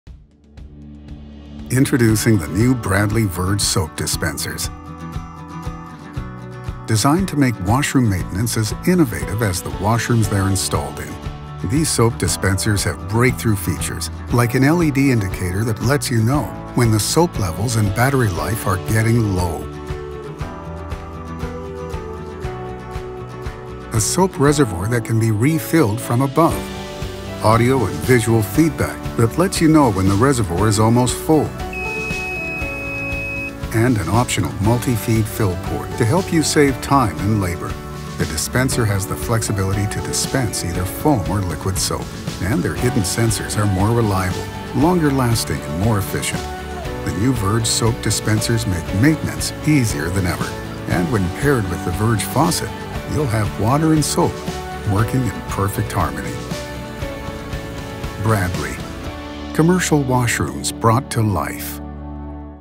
Introducing Verge Soap Dispensers
Verge top-fill soap dispensers make maintenance easier than ever. The smart sense system with LED indication lets maintenance staff know when soap and battery levels are low. Audible and visual alerts aid the refill process to prevent overfilling. And the new multi-feed soap system feeds up to six soap dispensers and is perfect for high traffic applications.